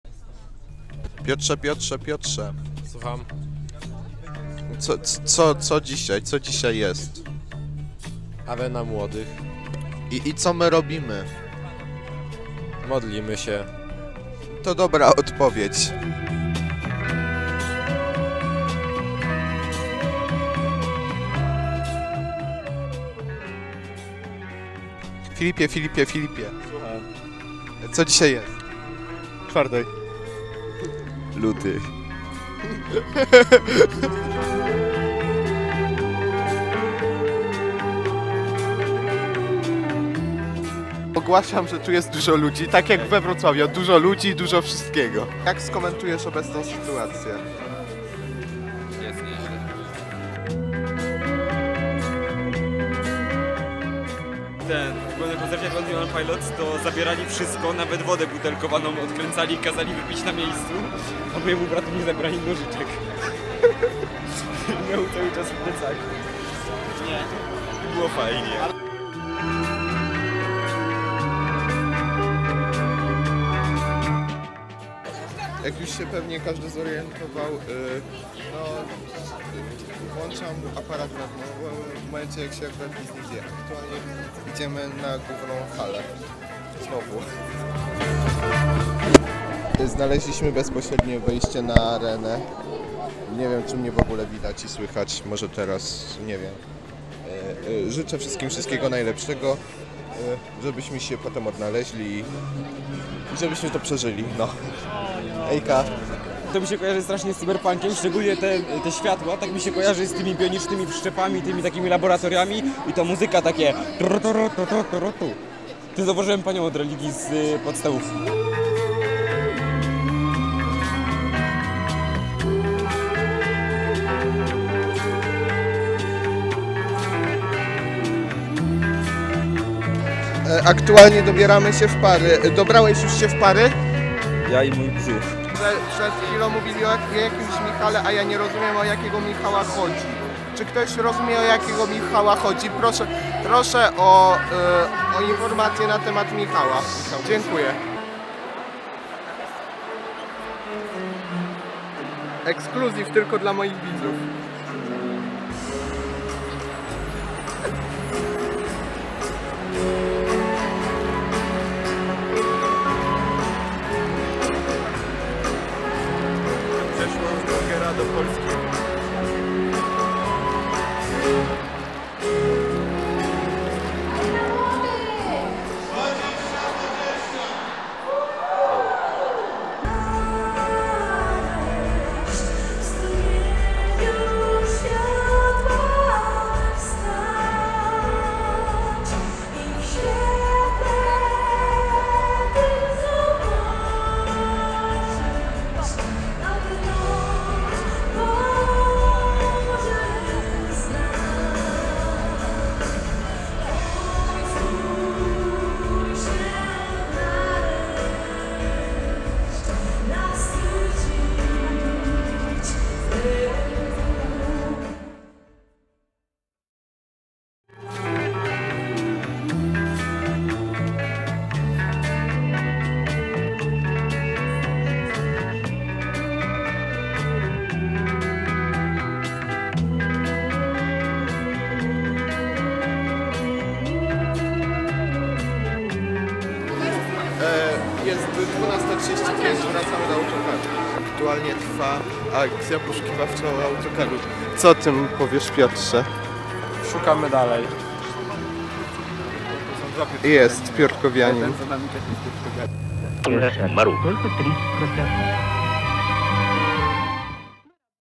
arena młodych 2020 wideorecenzja
wideorecenzja? nie! to tylko bardzo nieudana relecja z wchodzenia na arenę i losowe momenty. prawda jest taka, że miało to być na poważnie ale wyszło jak zwykle bo mi się nie zgrał cały materiał z karty, a tę wyczyściłem :(